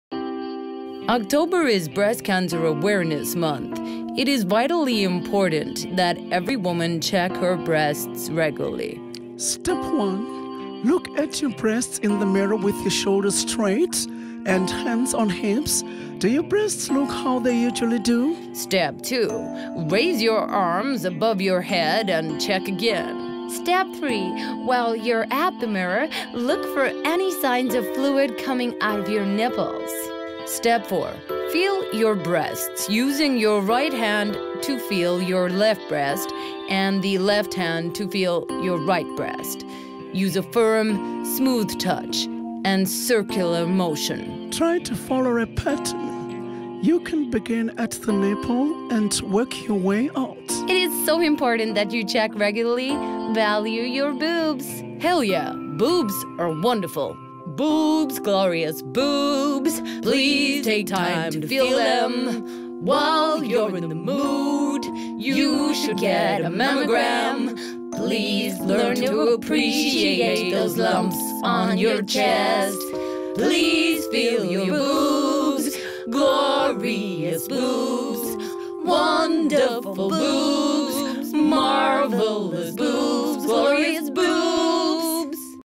Boobs Glorious Boobs
October is Breast Cancer Awareness month. It is vitally important that every woman check her breasts regularly! It is so important that you check regularly. Value your boobs! Thanks to Charlize Theron, Michelle Obama, Thuli Madonsela and Hillary Clinton for showing us how!